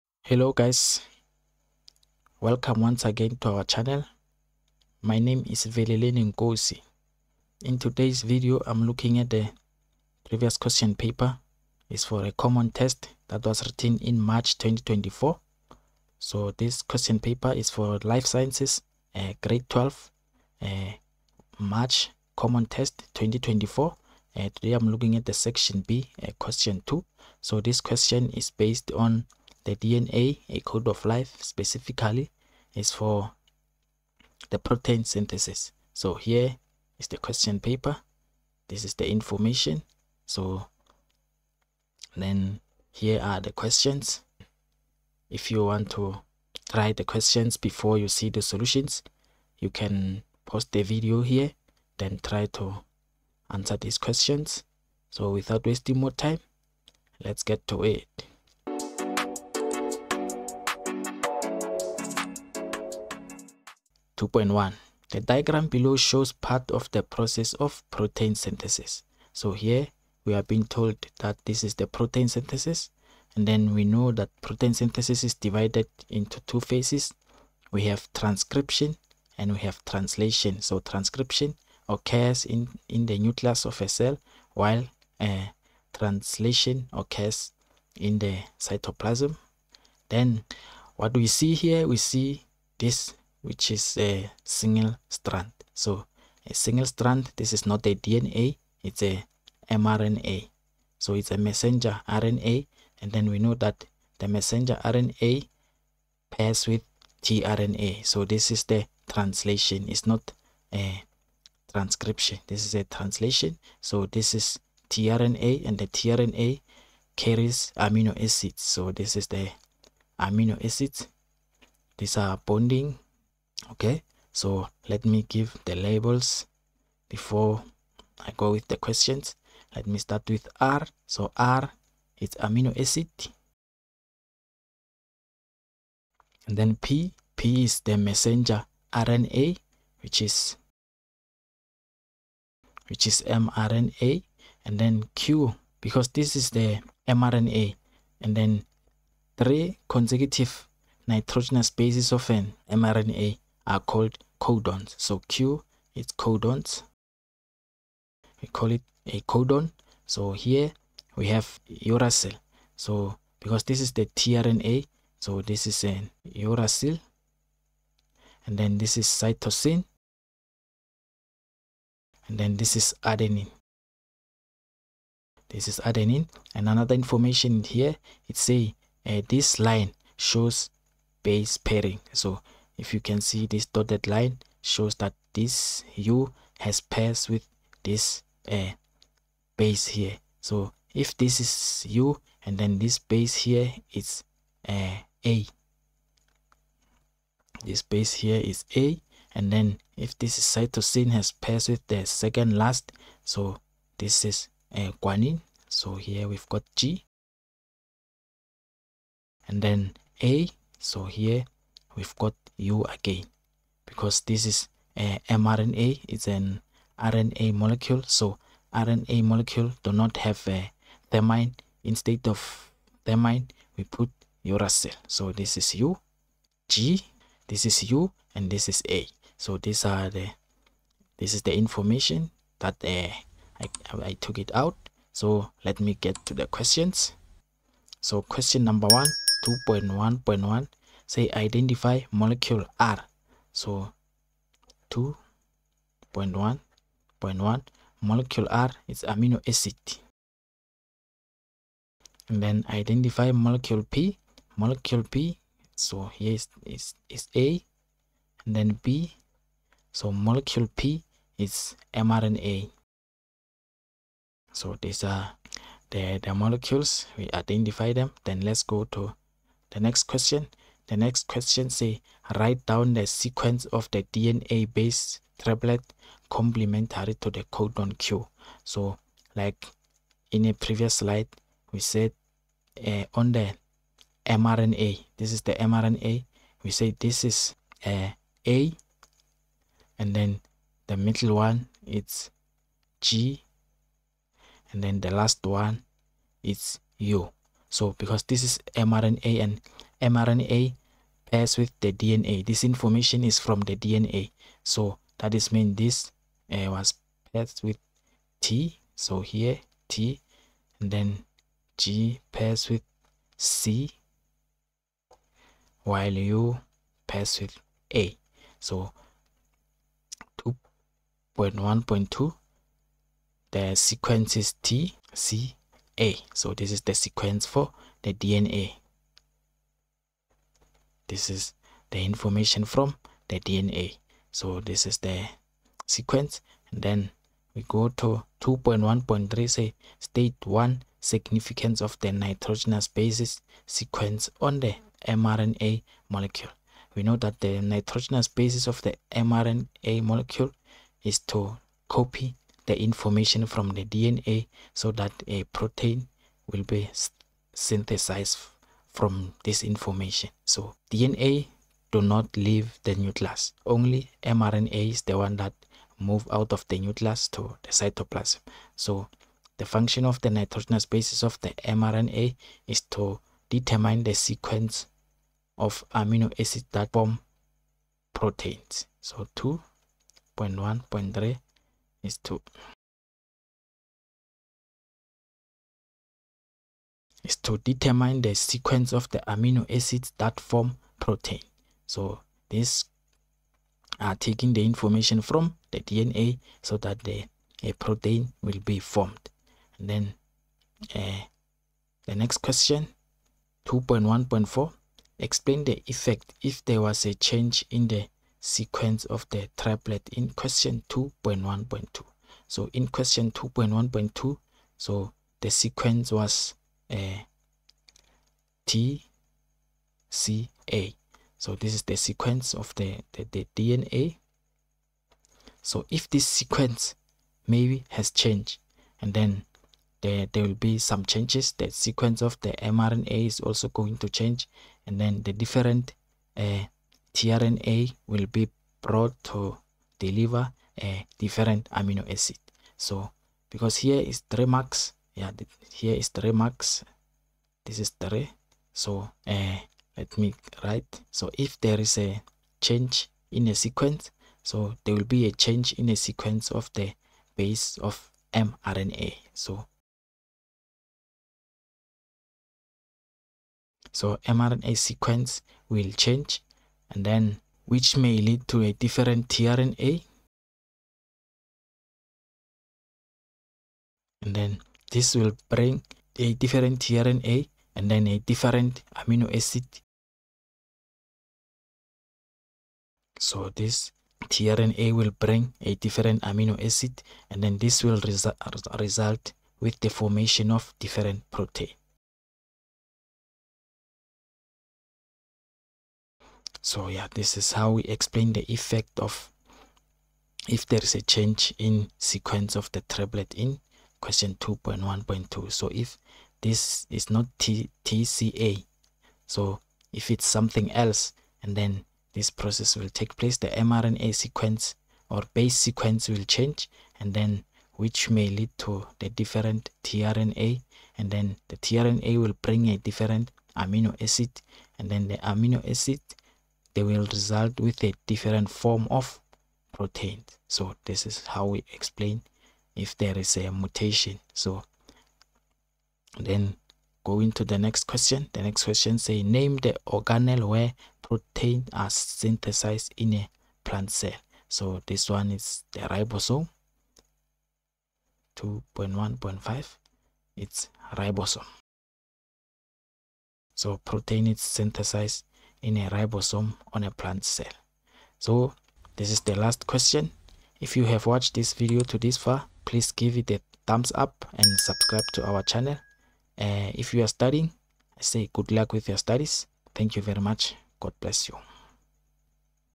March 2024 | Protein Synthesis | Life Science grade 12.
In this video we work on a common Test relating to the Protein synthesis, the paper was written in March 2024 KZN.